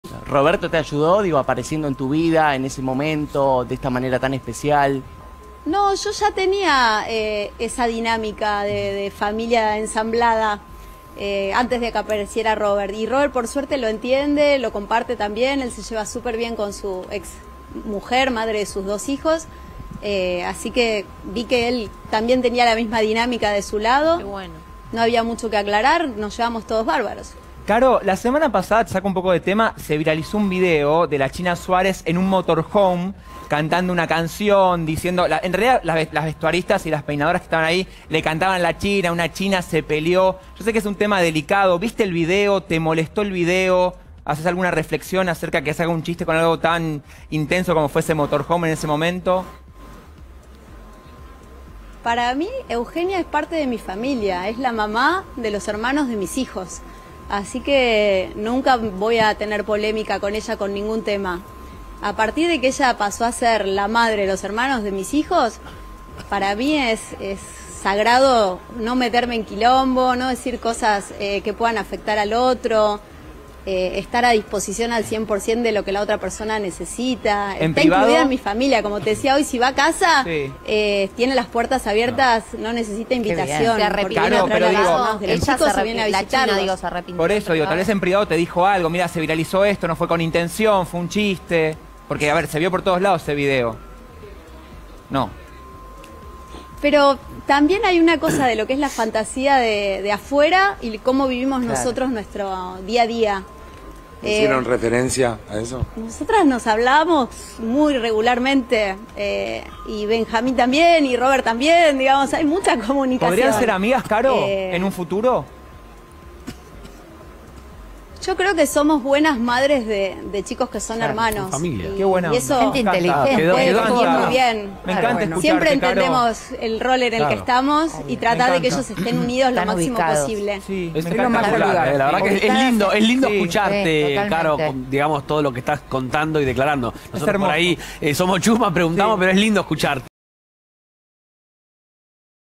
¿Qué opina Pampita sobre la China Suarez?
Las declaraciones de Carolina "Pampita" fueron que la China es como de su familia y tienen un muy buen vínculo entre ellas.